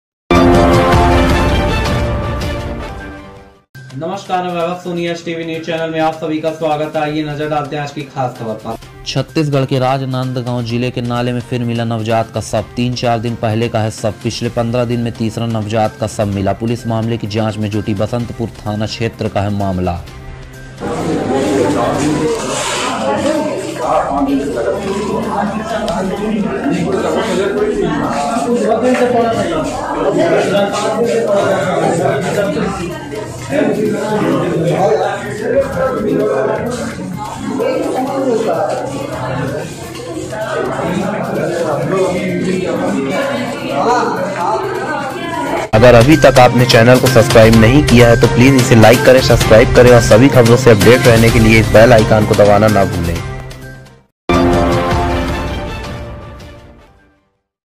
देखिए आज की ताजा खबर। Breaking news
छत्तीसगढ़

राजनांदगांव के नाले में फिर मिला नवजात का शव

रायपुर ।  राजनांदगांव के  नाले में फिर मिला नवजात का शव,तीन चार दिन पहले का है शव,पिछले 15 दिन में तीसरा नवजात का शव मिला,पुलिस मामले की जाँच में जुटी,बसंतपुर थाना क्षेत्र का मामला।